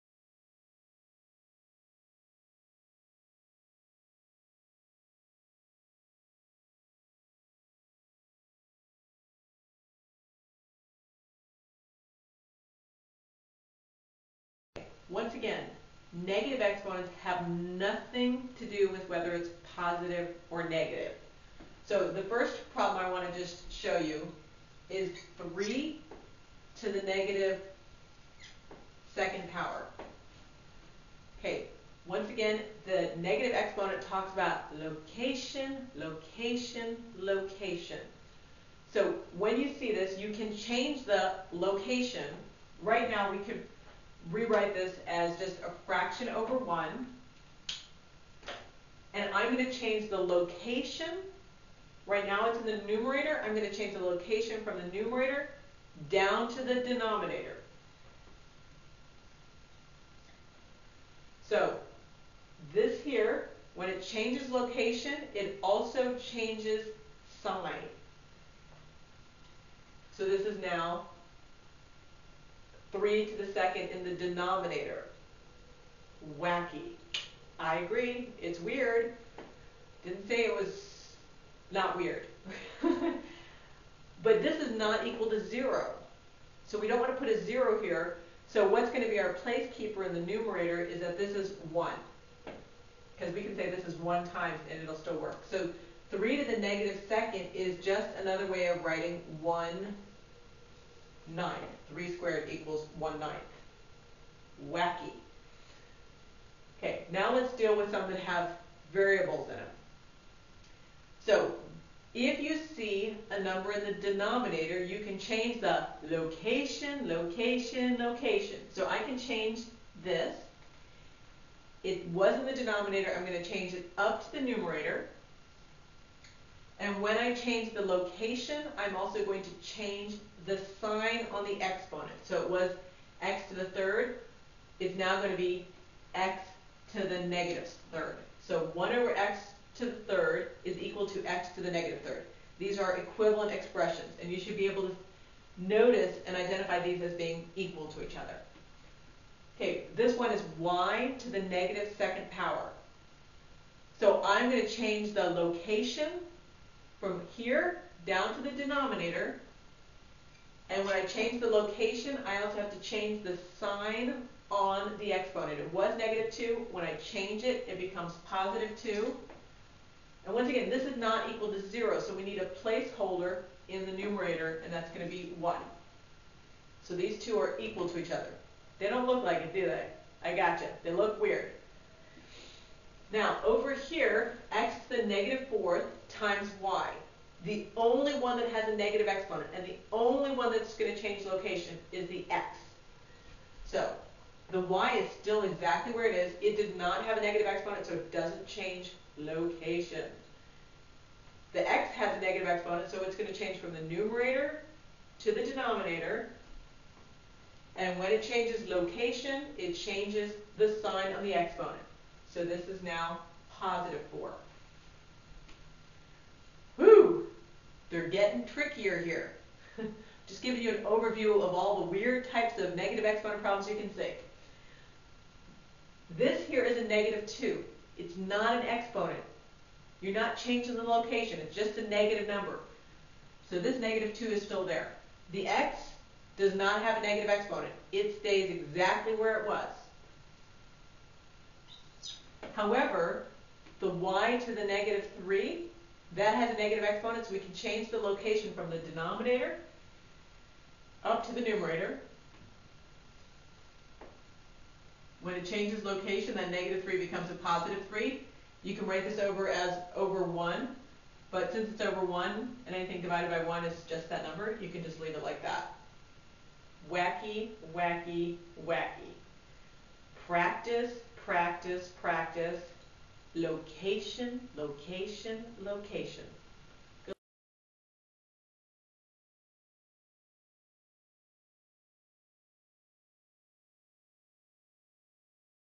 Negative Exponents II: The Wackiness Continues
for more math resources.

This is the sequel to Negative Exponents. Math can sometimes be complicated or confusing. Negative exponents are one of the wackiest topics. Don't get confused by the negative exponents. Watch this math video to get some tutoring.